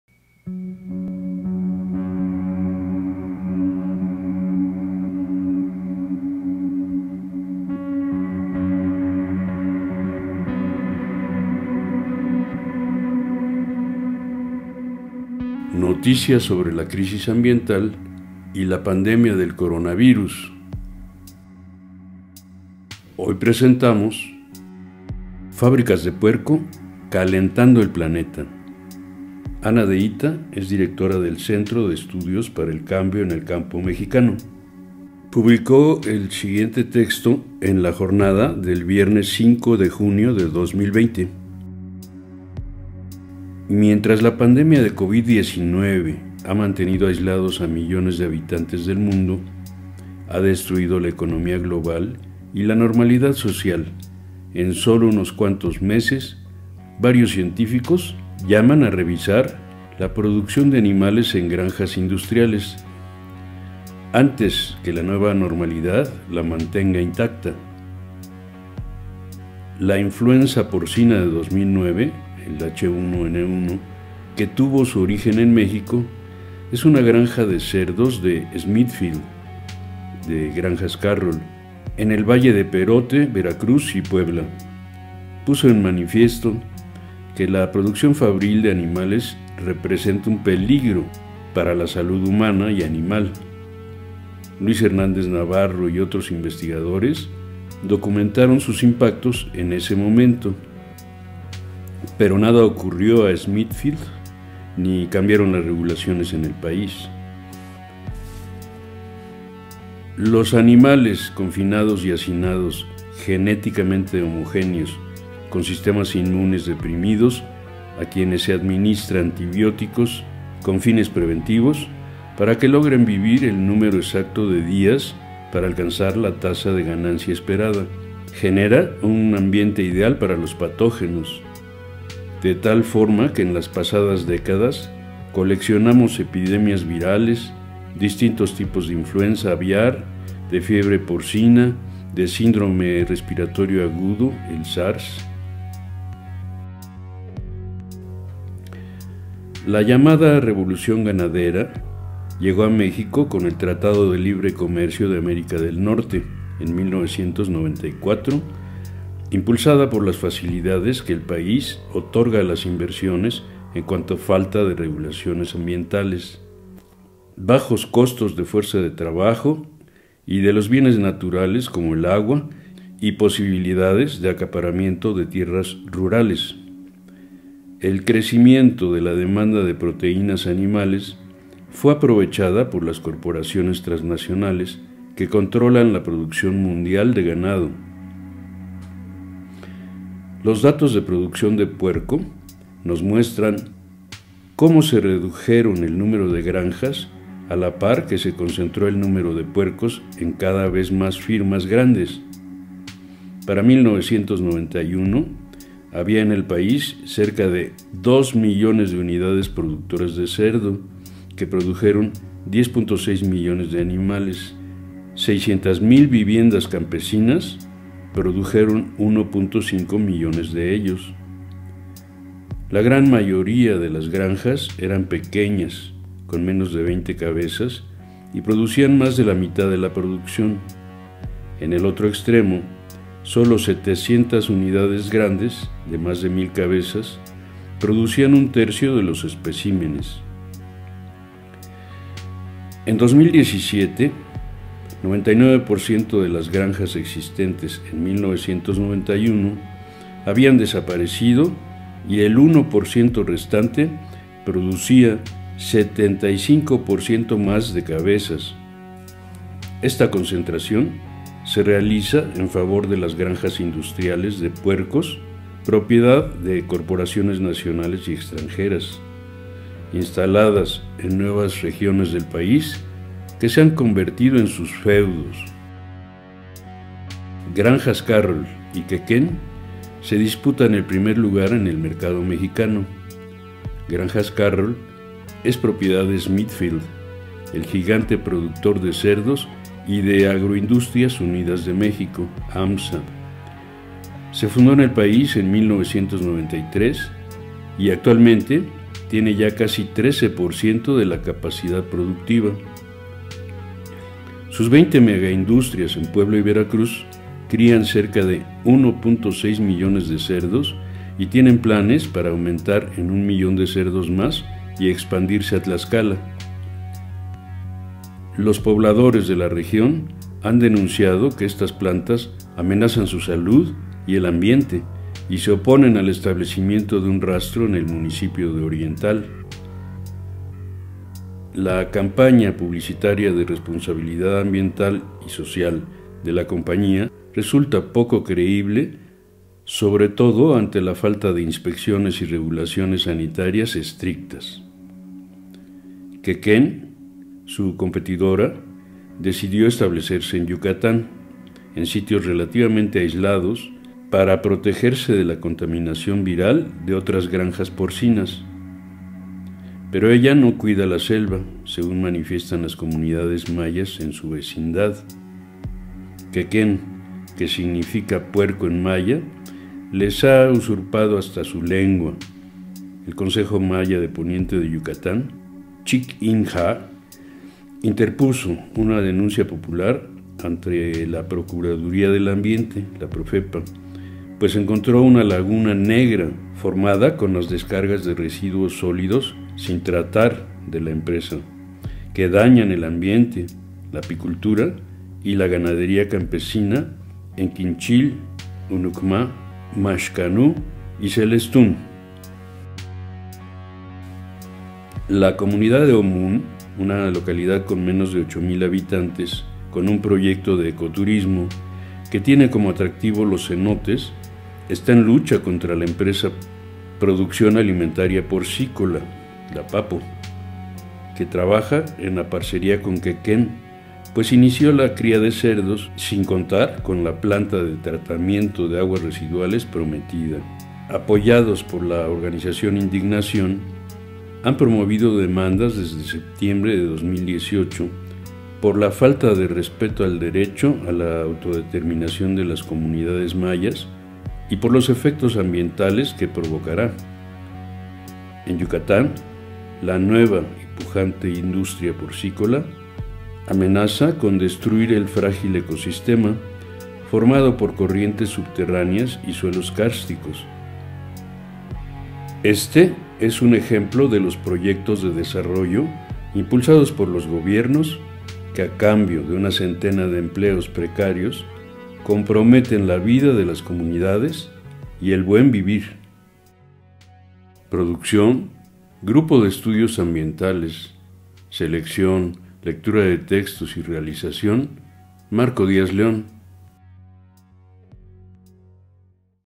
Fábricas de puerco: calentando el planeta / Noticias sobre la crisis ambiental y la pandemia...
Serie radiofónica:
Noticias sobre la crisis ambiental y la pandemia del coronavirus

Capítulo 15:
Fábricas de puerco: calentando el planeta

(Nota de Ana de Ita publicada en el periódico La Jornada el 5 de junio de 2020).

​Producción: Grupo de Estudios Ambientales, GEA.
Selección, lectura de textos y realización: Marco Díaz León.